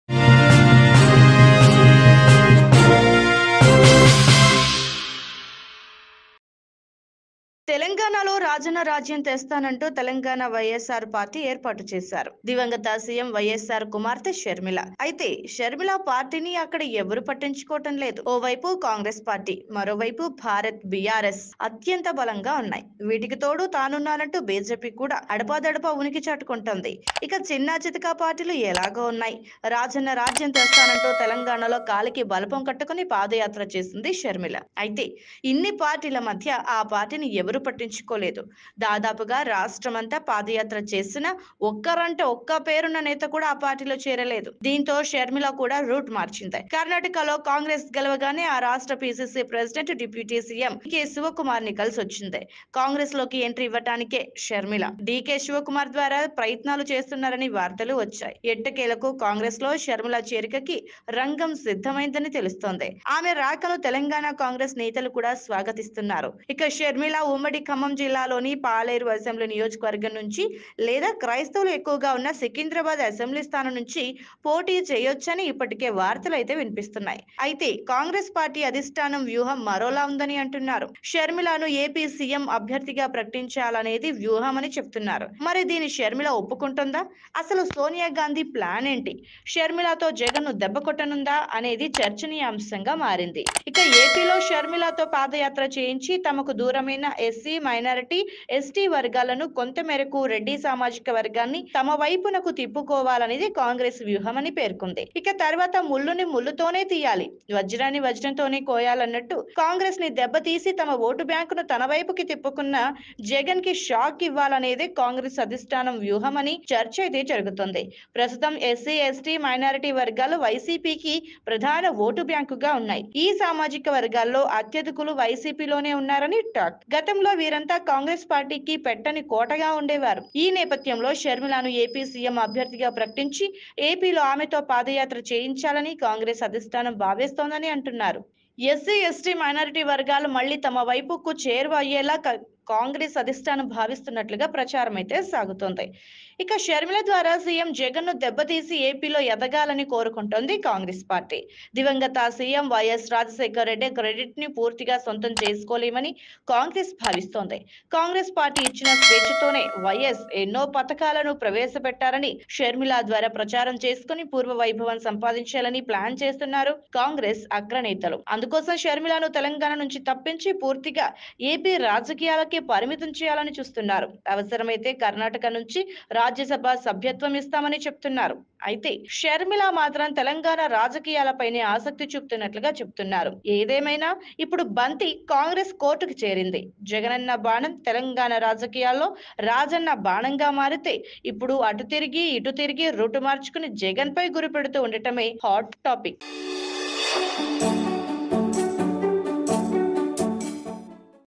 ఏపీ సీఎం అభ్యర్థిగా షర్మిల - YS Sharmila As AP Congress CM Candidate? | AP Politics | SocialPost Tv
SocialPost Shorts